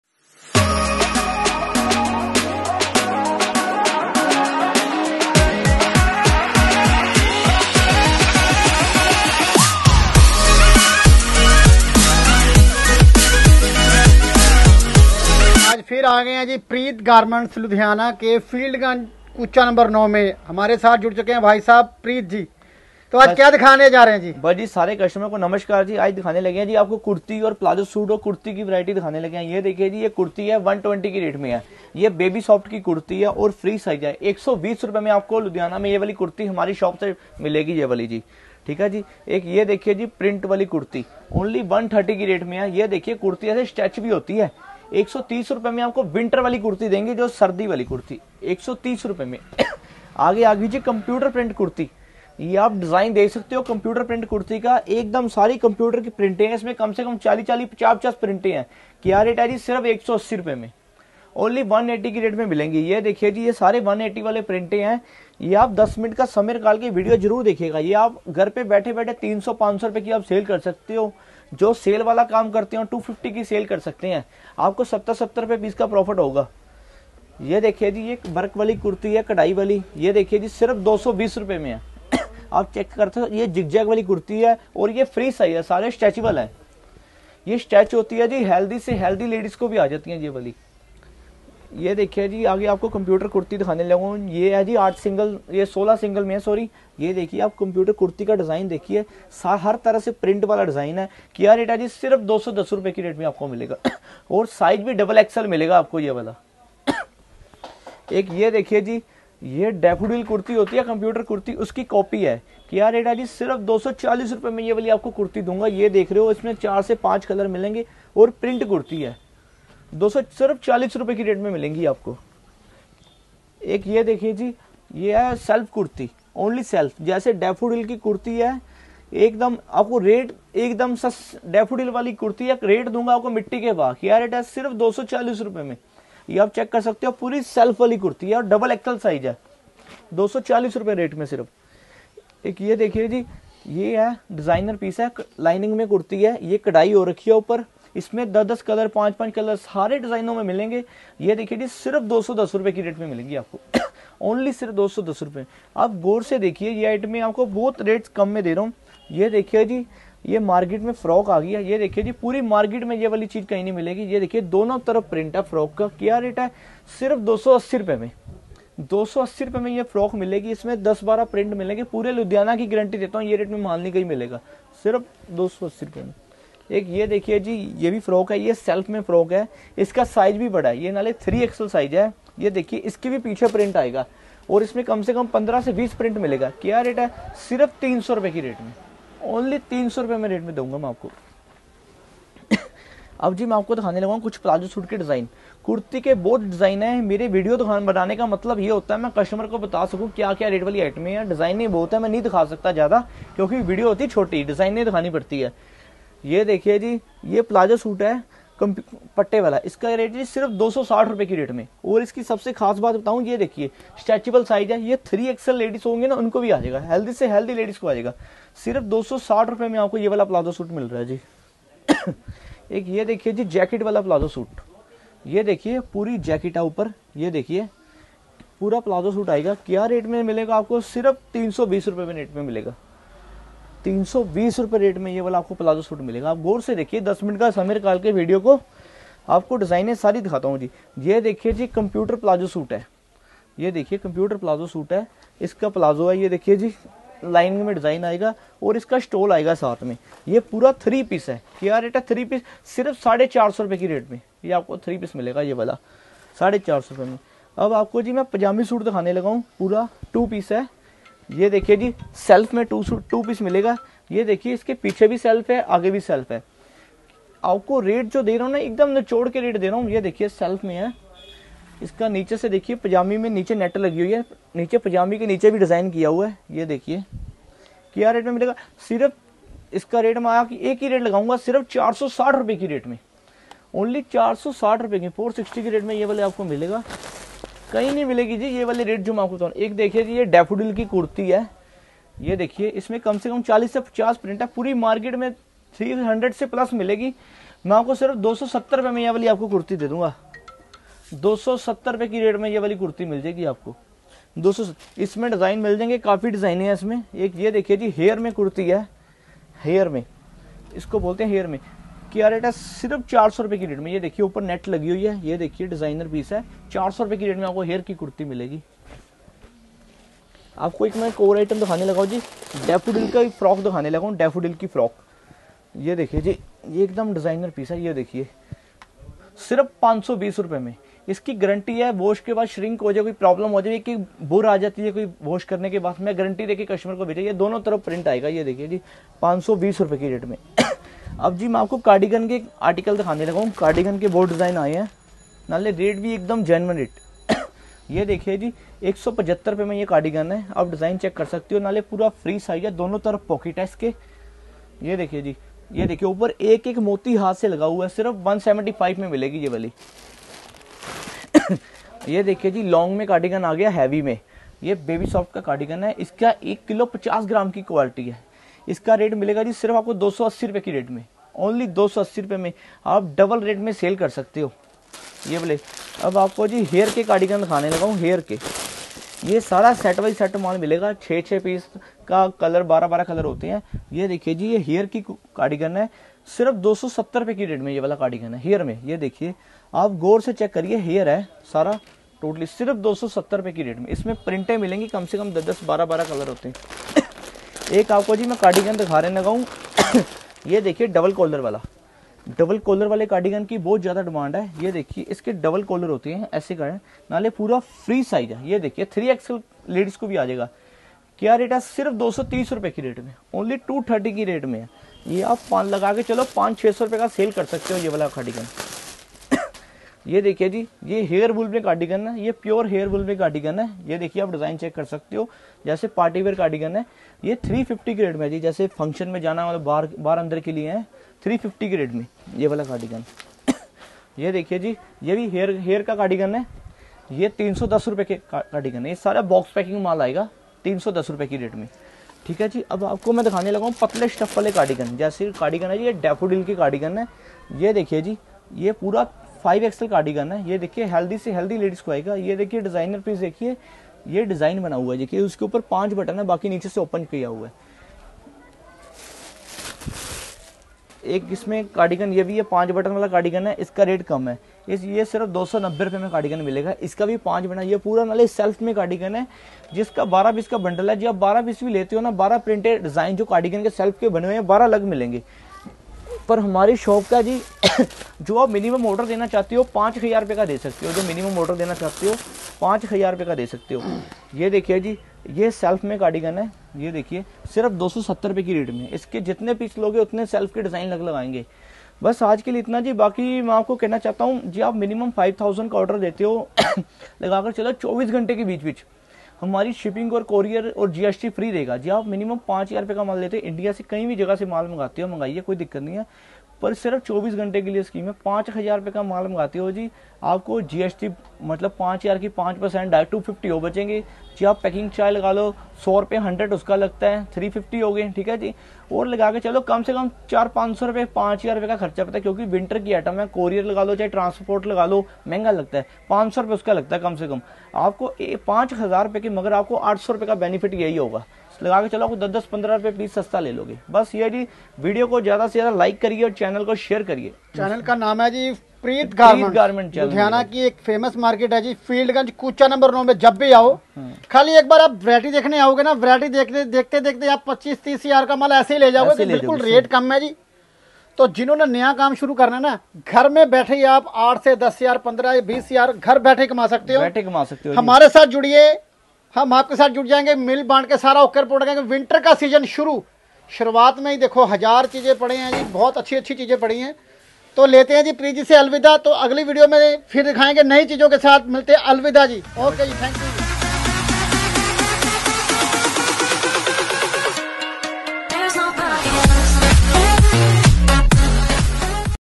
लुधियाना शहर में सबसे सस्ती गरम कुर्ती मात्र ₹130 से शुरू जबरदस्त सेल सेल सेल
.by preet Singh garmant

field Ganj Ludhiana Punjab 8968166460

by all ladies variety wolsale price video last Tak jaruar dekhiae video by Ludhiana top collection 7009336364

not responsibility any profit and loos thanks